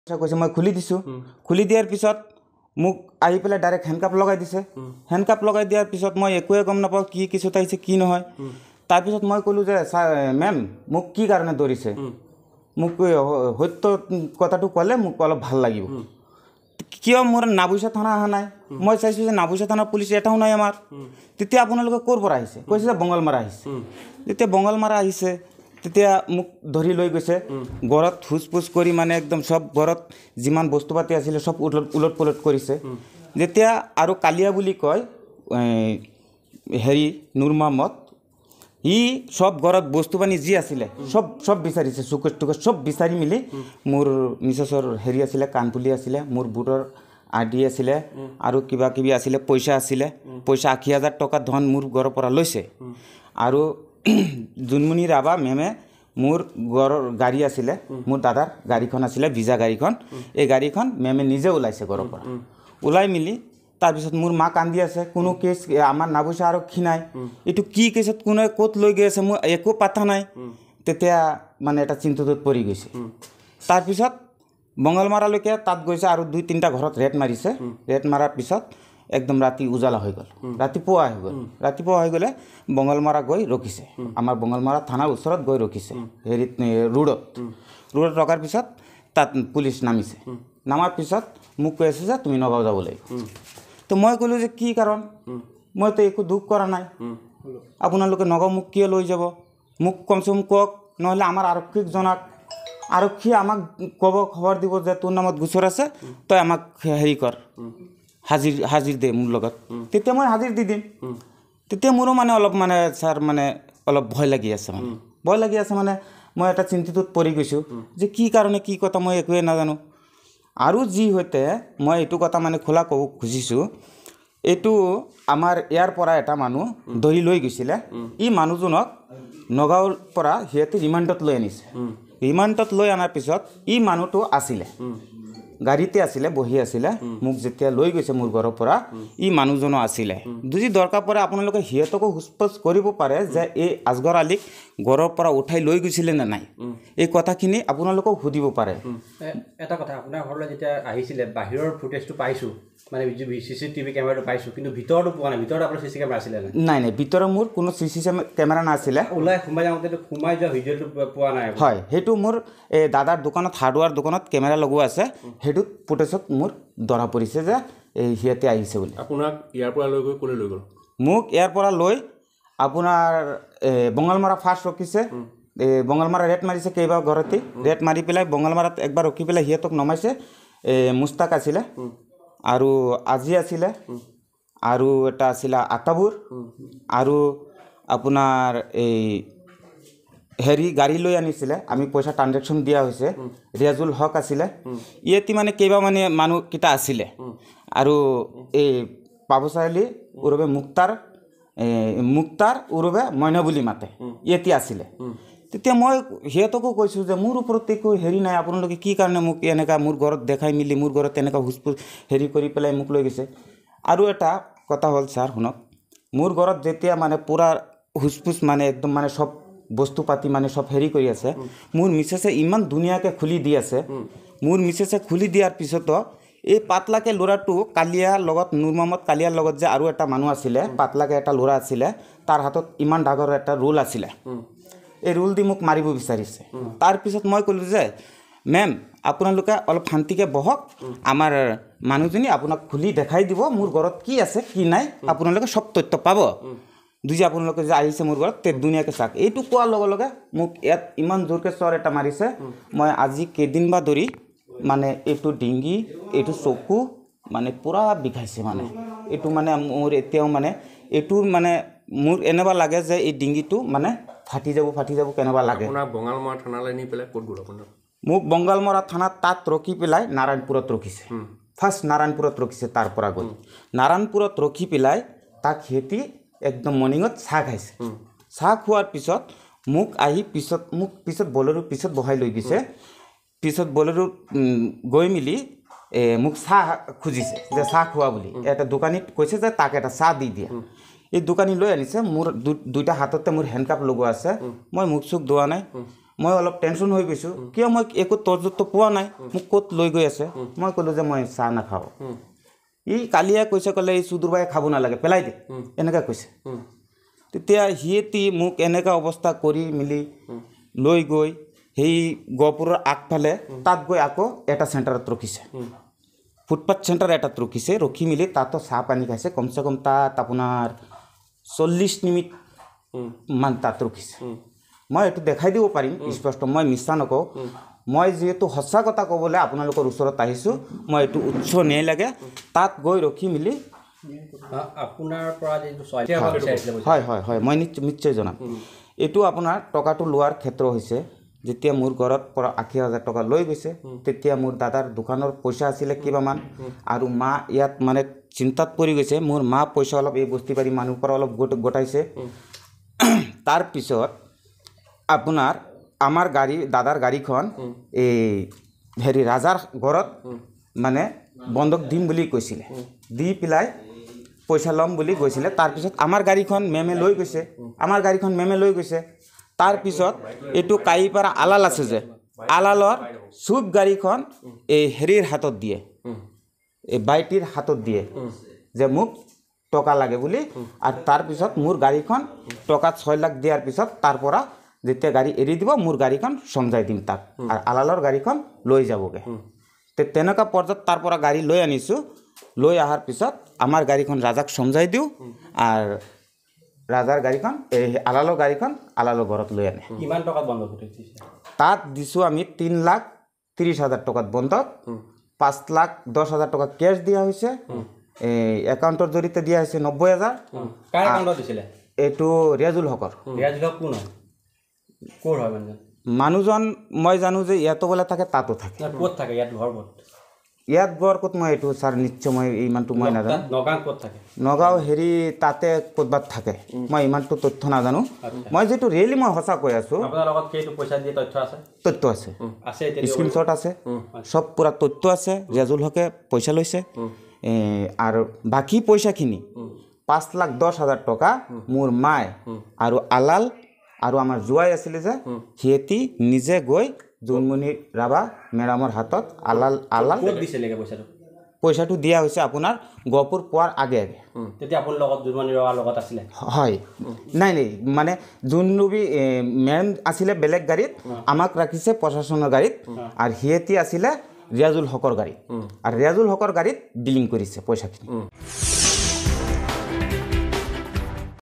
'ঘৰত সোধ-পোচ কৰি, ঘৰত যিমান বস্তু আছিল সকলো ওলট-পালট কৰিছে'
'ঘৰত সোধ-পোচ কৰি, ঘৰত যিমান বস্তু আছিল সকলো ওলট-পালট কৰিছে। মোৰ পত্নীৰ কাণফুলি, মোৰ ভোটৰ আই ডি আৰু পইচা আশী হাজাৰ টকা লৈ গৈছে'।